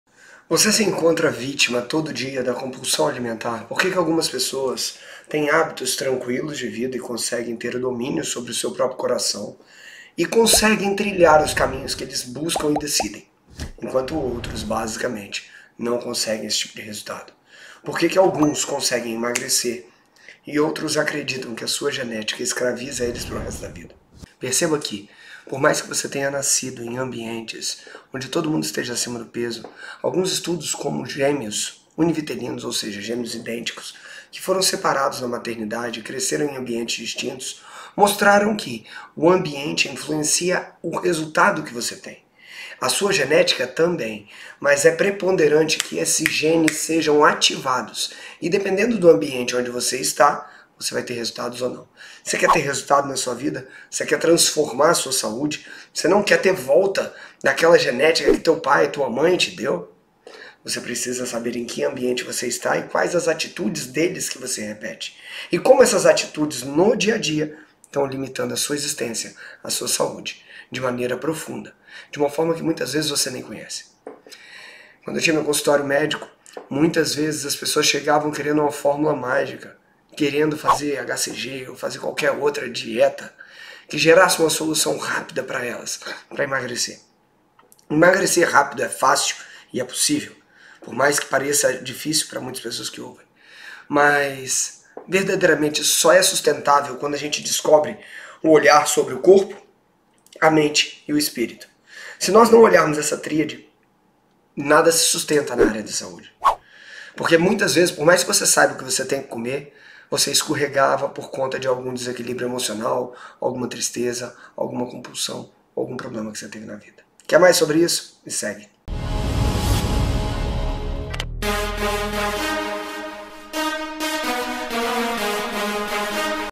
O QUE É E COMO RESOLVER A COMPULSÃO A ALIMENTAR | Dr Juliano Pimentel
COMPULSÃO ALIMENTAR, VOCÊ SOFRE DESTE MAL? | Dr Juliano Pimentel

Você se sente vítima da Compulsão Alimentar? Alguns estudos como gêmeos mostraramm que o ambiente influencia no resultado que você tem, o gene também!

Para transformar sua saúde, você precisa saber em que ambiente você está e e quais as atitudes dele que você repete e como isso afeta sua vida.

Emagrecer rápido é fácil e é possível, mas existe uma raiz que precisamos conhecer e tratar para que o sobrepeso não volte ainda pior!

Dr. Juliano Pimentel